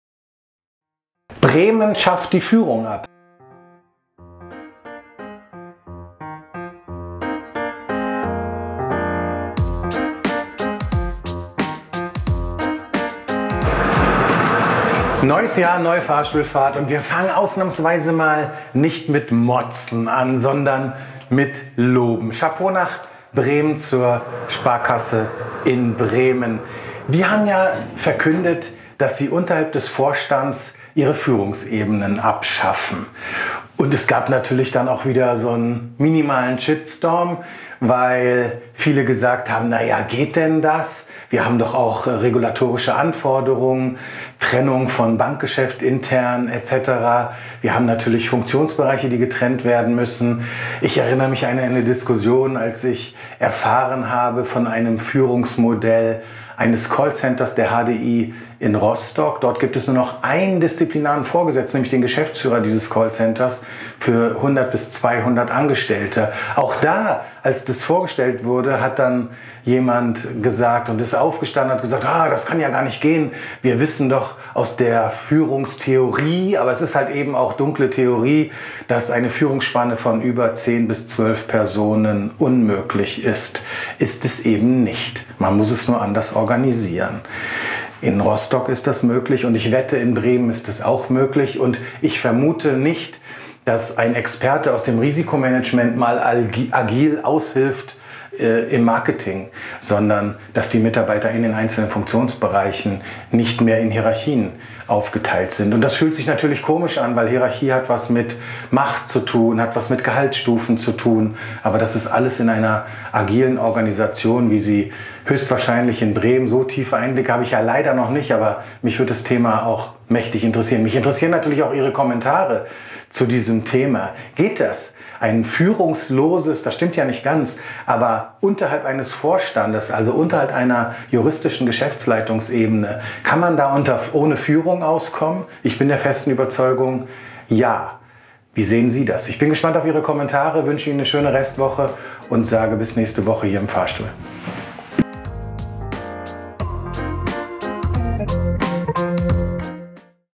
136: Auch Führung geht agil - FREITAGS im Fahrstuhl - Folge 136/2022
Gratulation sende ich zur "Die Sparkasse Bremen". Dort schafft man "Führung" unterhalb des Vorstandes ab.

Und klar, da kommen die theorieverliebten zu Wort, geht denn Führung ohne Führung? Gehen Spannen von mehr als zwölf Mitarbeitern. Was ist mit der regulatorischen Anforderung von Funktionstrennung?

Kann man Mitarbeiter ohne Karriere binden? Fallen Führungskräfte ohne Dienstgrad in ein tiefes Loch?

Was sagt "Bremen" denn dazu?